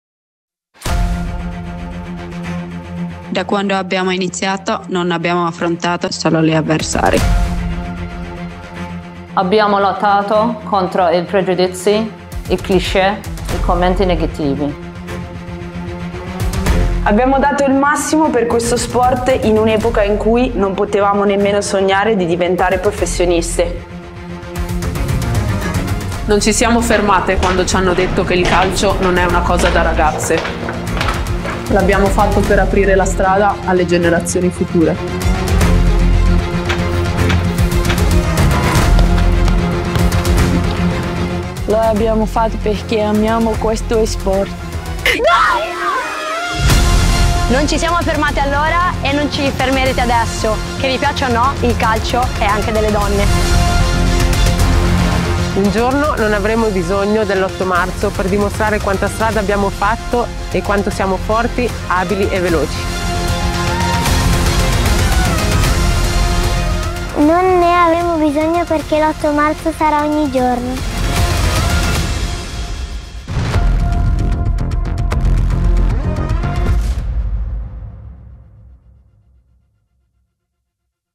InspireInclusion: il messaggio dell’U.C. Sampdoria
Lottare per i propri diritti, contrastando stereotipi, pregiudizi e commenti negativi. Lottare per inseguire la propria passione per il calcio, dimostrando che il gioco più bello del mondo è anche delle donne. Un messaggio forte e diretto quello lanciato dalla Sampdoria per l'8 marzo, Giornata internazionale della donna. Una ricorrenza che quest'anno ha come tema InspireInclusion a voler sottolineare l'importanza della diversità e dell'emancipazione nel calcio e nello sport così come in tutti gli aspetti della società e ci invita a concentrarci sull'importanza di includere tutte le donne, indipendentemente dalla loro età o provenienza.